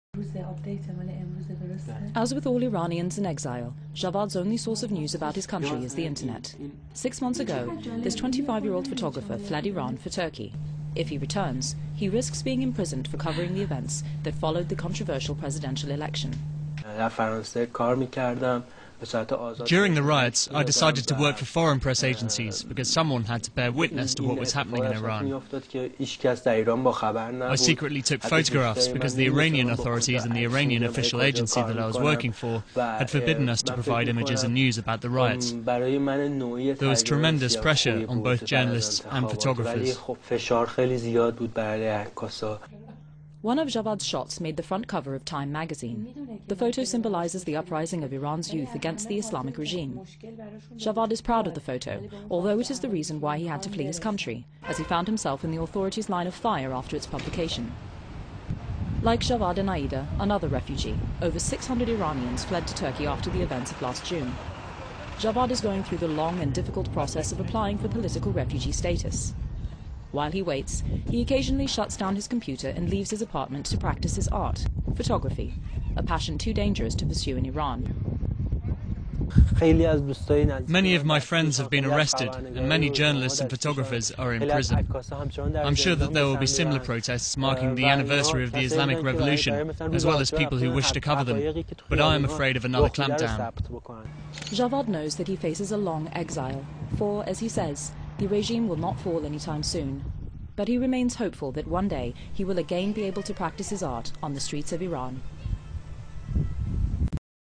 France24 February 12: Iran opposition: a long exile in Turkey
IRAN ISLAMIC REVOLUTION ANNIVERSARY - As with all Iranians in exile, Javad's only source of news about his country is the internet. Six months ago, this 25-year-old photographer fled Iran for Turkey. If he returns, he risks being imprisoned for covering the events that followed the controversial presidential election.